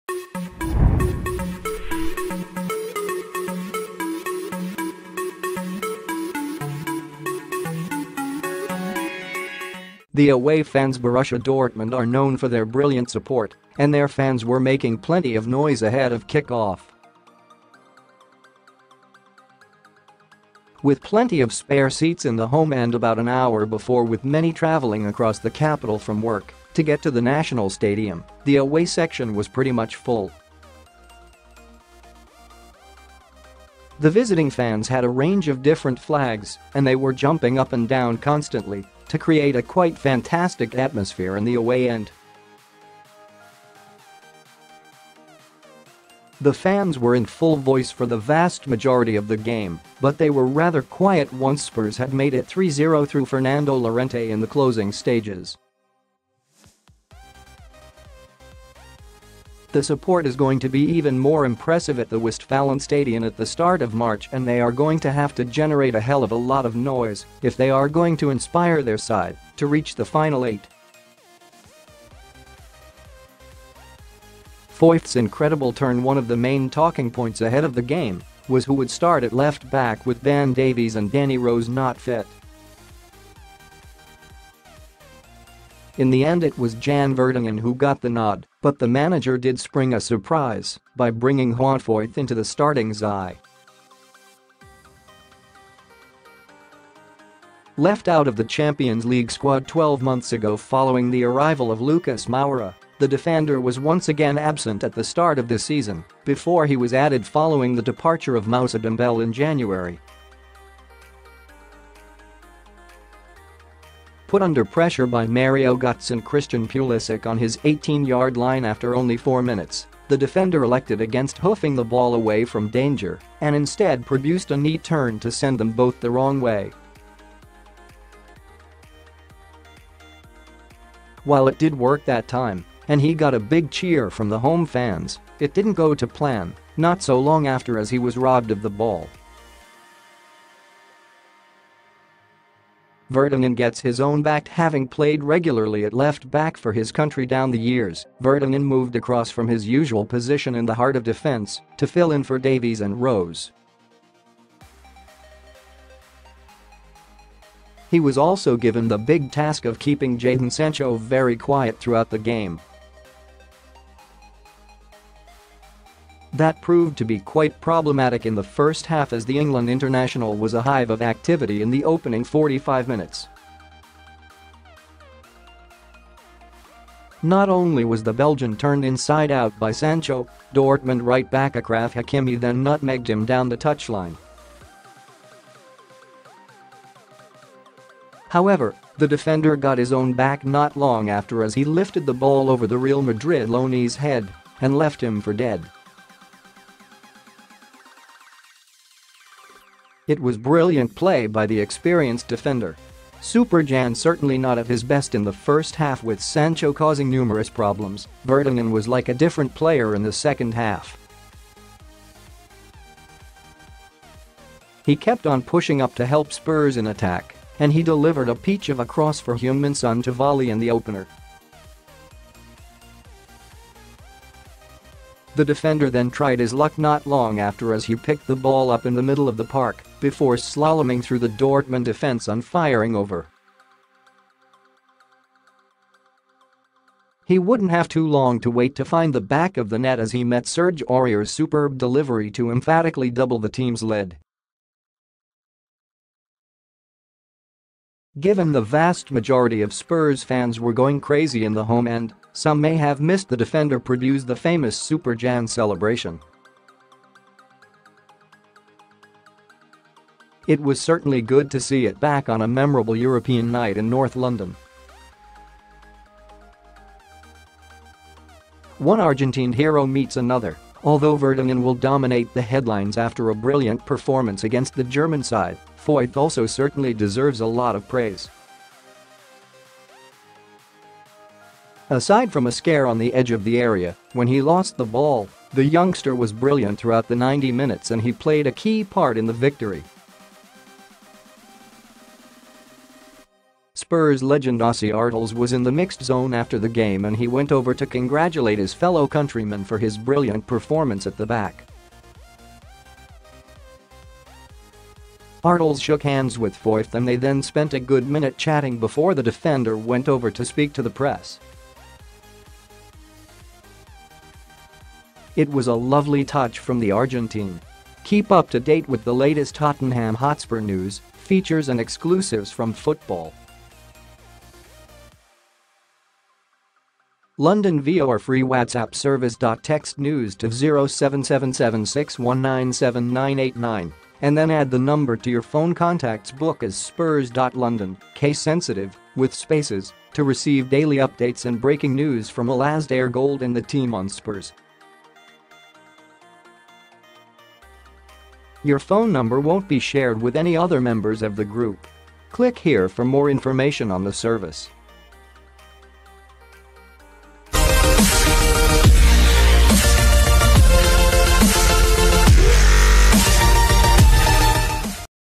Sport TV - Vertonghen's revenge, Foyth's turn & Dortmund fans - Spurs moments missed
The away fans   Borussia Dortmund are known for their brilliant support and their fans were making plenty of noise ahead of kick-off.  With plenty of spare seats in the home end about an hour before with many travelling across the capital from work to get to the national stadium, the away section was pretty much full.  The visiting fans had a range of different flags and they were jumping up and down constantly to create a quite fantastic atmosphere in the away end.  The fans were in full voice for the vast majority of the game, but they were rather quiet once Spurs had made it 3-0 through Fernando Llorente in the closing stages.    The support is going to be even more impressive at the Westfalenstadion at the start of March and they are going to have to generate a hell of a lot of noise if they are going to inspire their side to reach the final eight.   Foyth's incredible turn   One of the main talking points ahead of the game was who would start at left-back with Ben Davies and Danny Rose not fit.  In the end it was Jan Vertonghen who got the nod, but the manager did spring a surprise by bringing Juan Foyth into the starting XI.  Left out of the Champions League squad 12 months ago following the arrival of Lucas Moura, the defender was once again absent at the start of this season before he was added following the departure of Mousa Dembele in January.     Put under pressure by Mario Gotze and Christian Pulisic on his 18-yard line after only four minutes, the defender elected against hoofing the ball away from danger and instead produced a neat turn to send them both the wrong way.  While it did work that time and he got a big cheer from the home fans, it didn't go to plan not so long after as he was robbed of the ball.   Vertonghen gets his own back   Having played regularly at left-back for his country down the years, Vertonghen moved across from his usual position in the heart of defence to fill in for Davies and Rose.  He was also given the big task of keeping Jadon Sancho very quiet throughout the game.  That proved to be quite problematic in the first half as the England international was a hive of activity in the opening 45 minutes.   Not only was the Belgian turned inside out by Sancho, Dortmund right-back Achraf Hakimi then nutmegged him down the touchline.  However, the defender got his own back not long after as he lifted the ball over the Real Madrid loanee's head and left him for dead.  It was brilliant play by the experienced defender.   Super Jan   Certainly not at his best in the first-half with Sancho causing numerous problems, Vertonghen was like a different player in the second half.  He kept on pushing up to help Spurs in attack and he delivered a peach of a cross for Heung-Min Son to volley in the opener.     The defender then tried his luck not long after as he picked the ball up in the middle of the park before slaloming through the Dortmund defence and firing over.  He wouldn't have too long to wait to find the back o